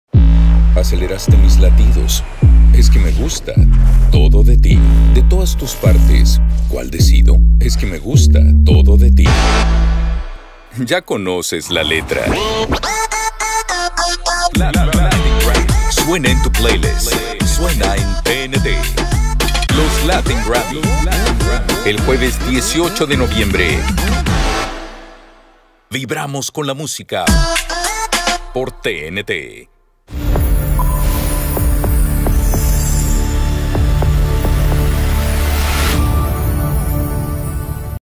Los Latin GRAMMY 2021
El artista puertorriqueño Rauw Alejandro ha logrado 3 nominaciones en los premios anuales del Latin Grammy Awards ¿logrará llevarse todas? Sigue la premiación en vivo por TNT a través de Dish